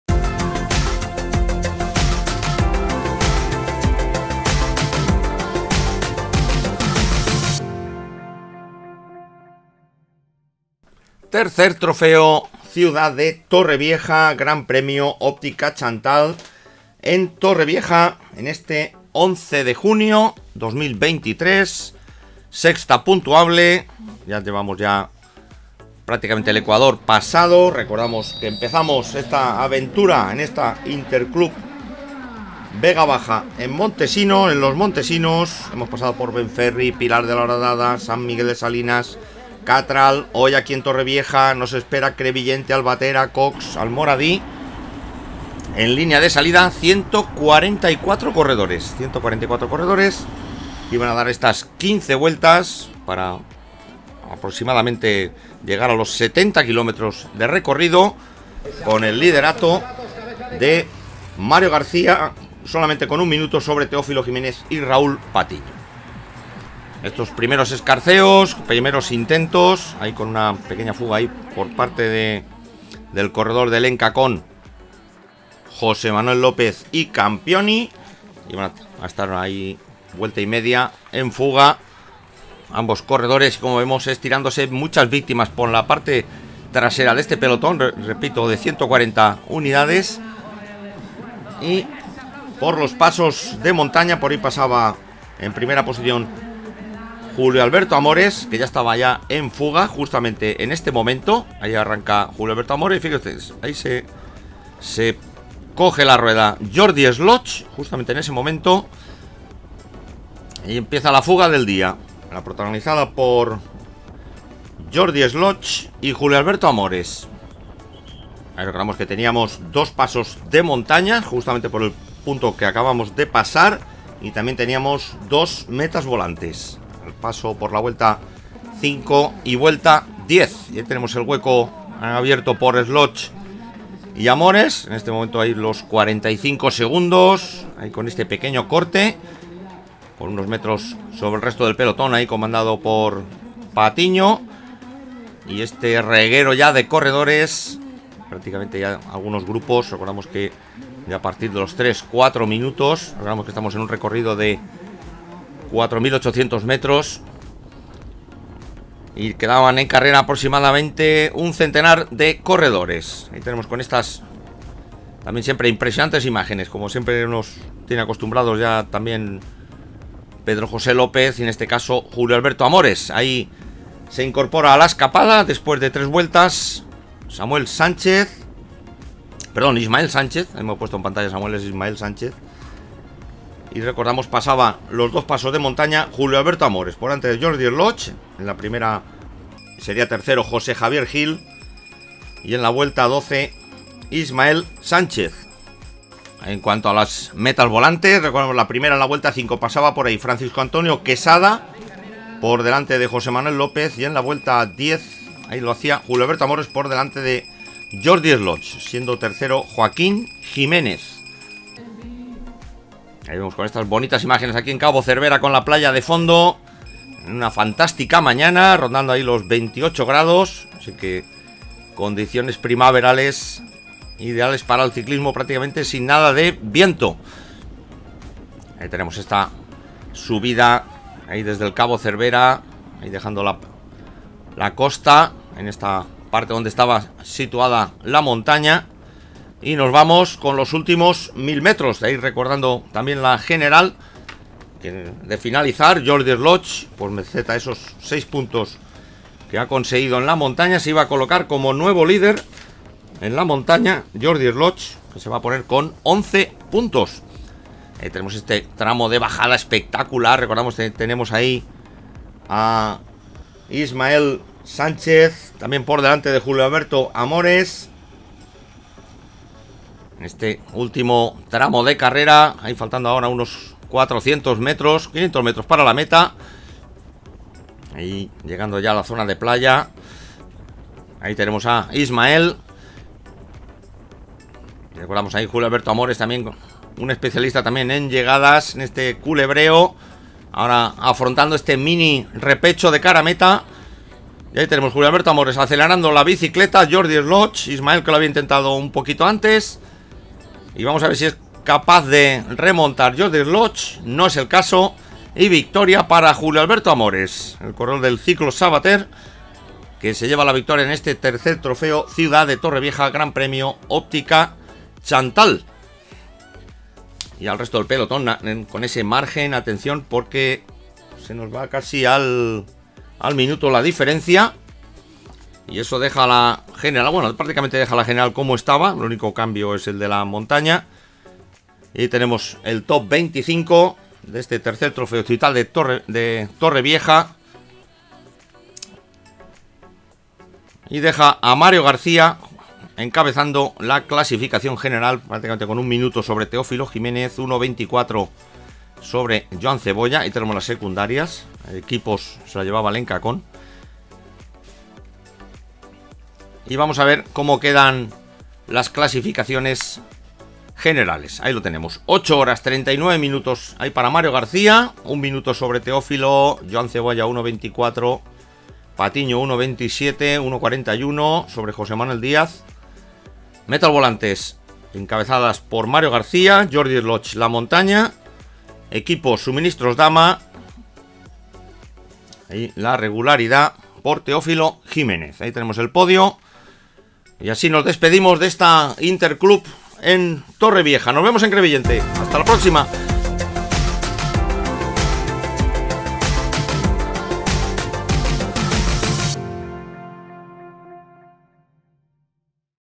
XXVIII Interclub Vega Baja Torrevieja 11-6-2023 Ciclismo 4K
XXIX Interclub Vega Baja Torrevieja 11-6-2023 Ciclismo 4K
Resumen 6ª cita de la XXVIII Interclub Vega Baja en Torrevieja sobre 15 vueltas de 4800m y 68 Km.
Tops:
AMORES, Julio Alberto
SLOOTJES, JORDI
SANCHEZ, Ismael
CAMPIONI, Mateo
JIMENEZ, Teofilo
GALLEGO, Moises
LOPEZ, Pedro Jose
LOPEZ, Jose Manuel
QUESADA, Julio
DIAZ, Jose Manuel
CEBOLLA, Joan
CALATAYUD, Sergio
DOMENE, Antonio
RUIZ, Sergio
FERNANDEZ, Francisco
AMOROS, Jorge
BERNABE, Anselmo
VAZQUEZ, Jose
GIMENEZ, Joaquin
GUILLEN, Jose Fco.
QUESADA, Fco. Antonio
GARCIA, Tomas
GARCIA, Ismael
RUFETE, Pablo
LOPEZ, Francisco
ORRICO, Ivan
GIL, Jose Javier
DOMENE, Ramon
MEDRANO, Antonio
LLOPIS, Ismael
CARRION, Israel
BAREA, FELIPE
FILIU, Juan Francisco
BARAJAS, Juan Antonio
GARCIA, Adrian
NAVARRO, Juan Carlos
VICTORIO, Joaquin
PARRA, Miguel
OTTESEN, JONATHAN